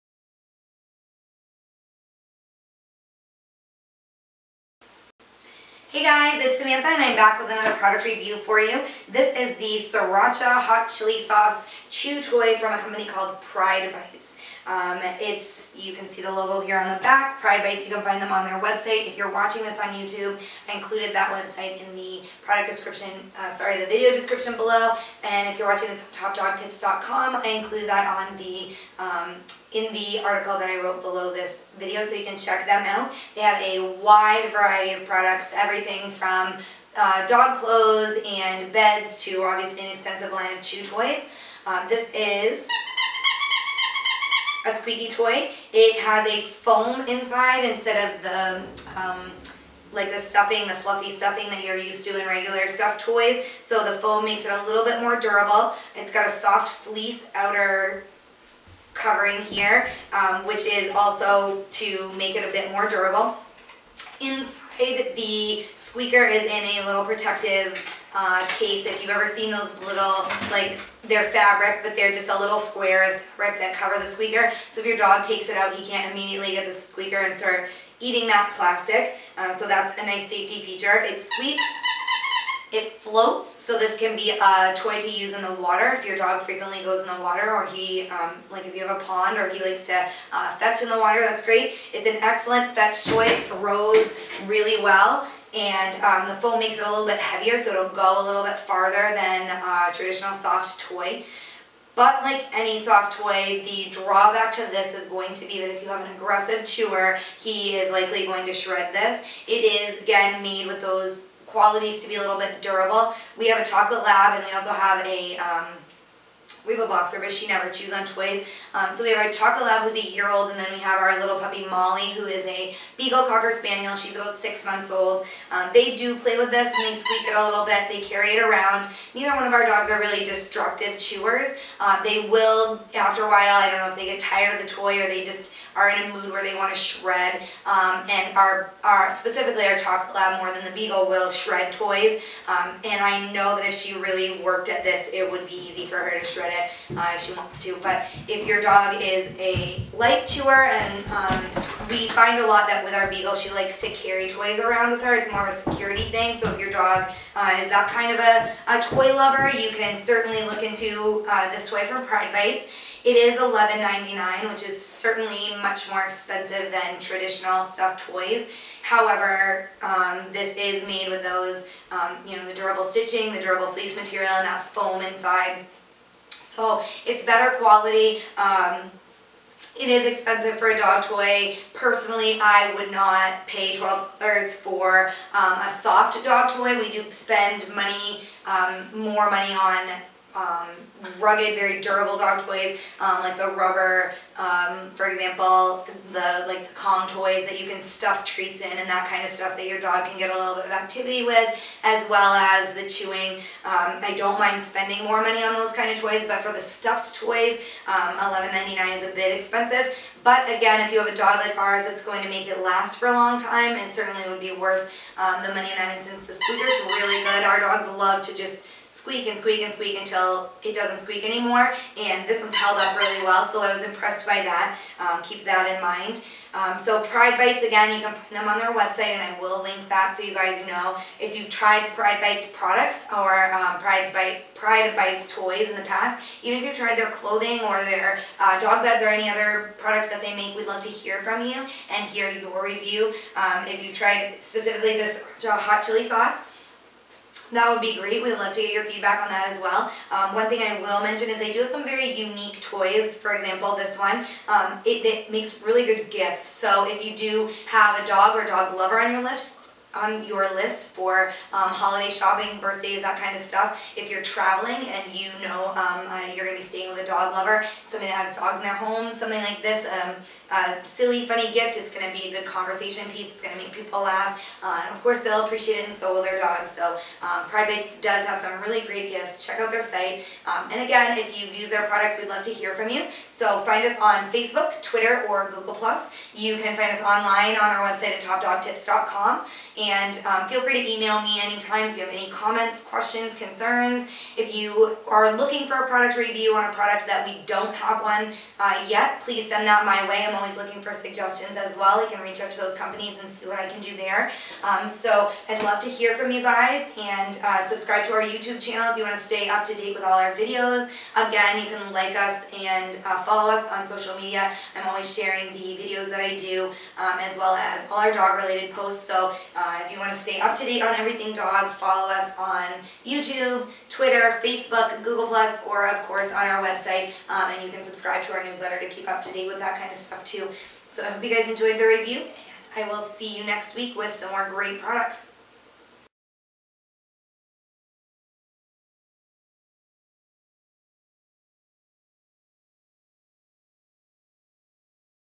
PrideBites Sriracha Dog Toy Review
Find out what Samantha, and her dogs, think of this PrideBites Sriracha chew toy.

"Once in a while they will start a game of tug that results in a ripped toy, but for the most part dog toys in our home last for months. This Sriracha dog toy from PrideBites was no exception. Our girls love to play with it, and it's very versatile. They've yet to destroy it, but I have no doubt that an aggressive chewer could destroy this dog toy in under a minute."